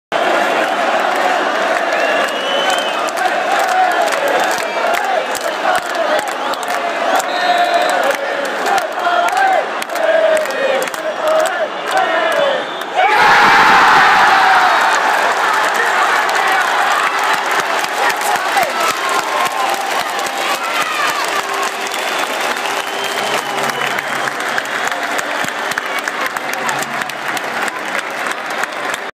Jigs Penalty Against Newcastle United
Jigs penalty against Newcastle United in a pre-season friendly @ Ibrox. Also known as the Sports Direct Friendly.